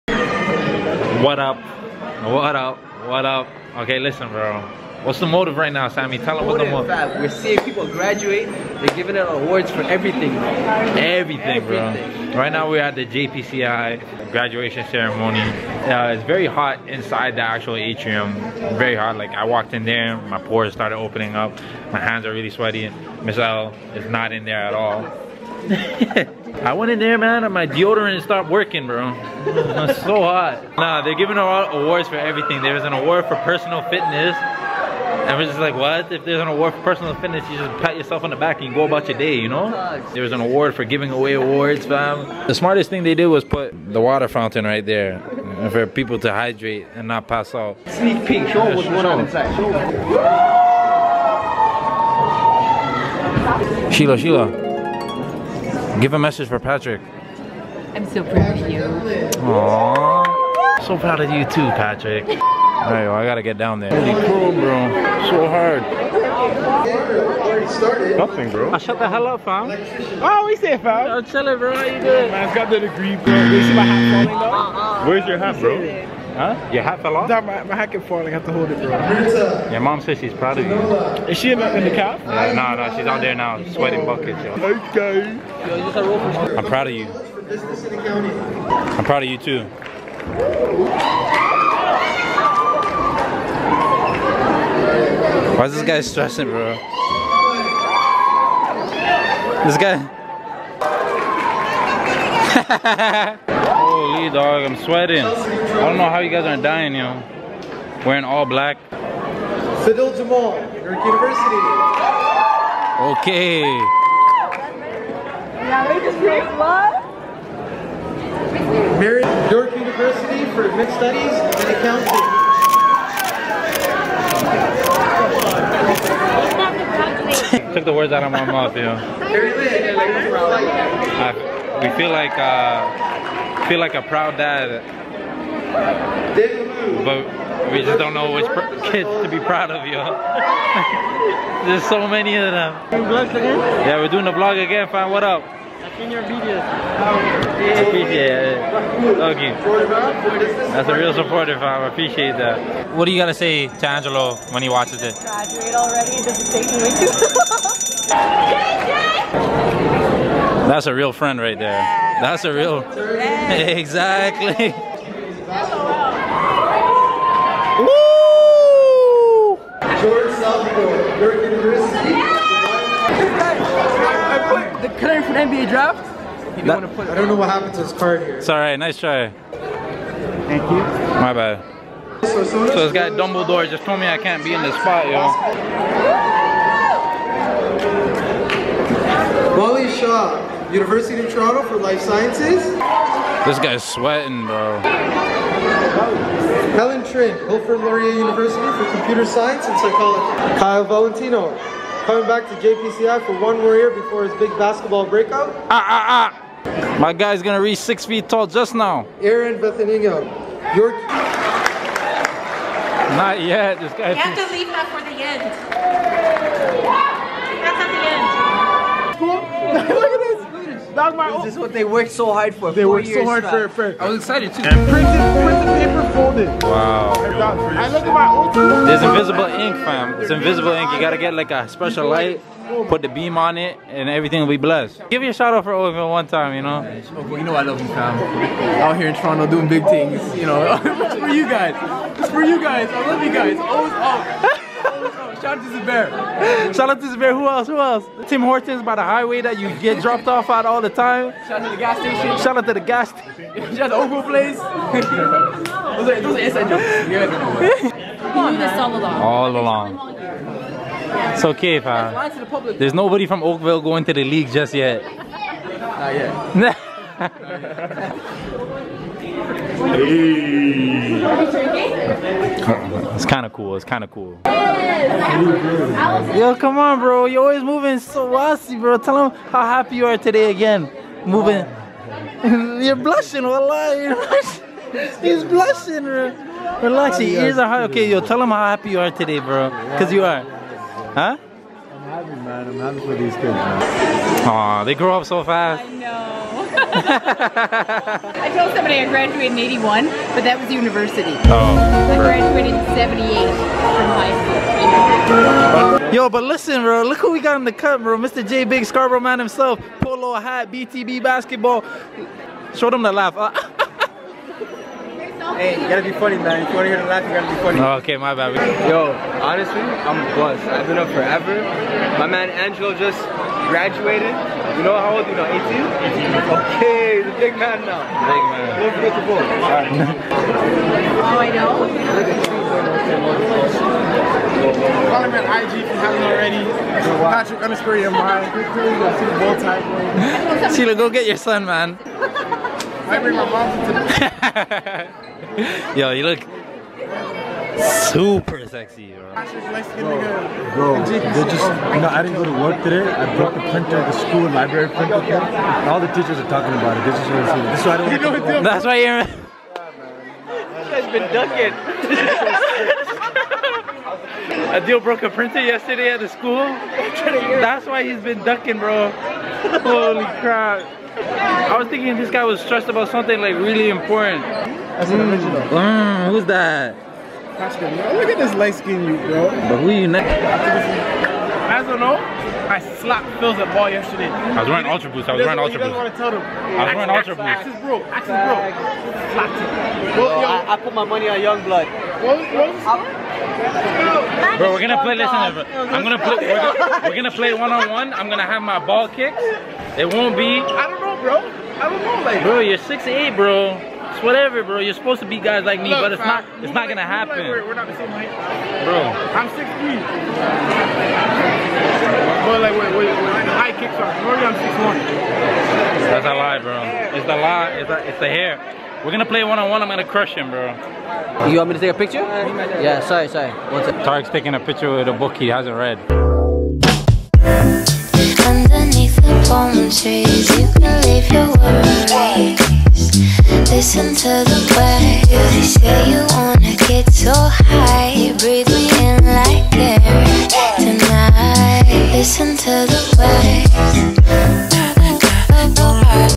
JPCI GRADUATION WAS ACTUALLY LIT!!!
VLOG #82 - I love to see my young brothers from Toronto Basketball Academy graduate! But Patrick actually thinks he could beat me 1v1??? ARE YOU GOOD IN YOUR HEAD FAM???!!!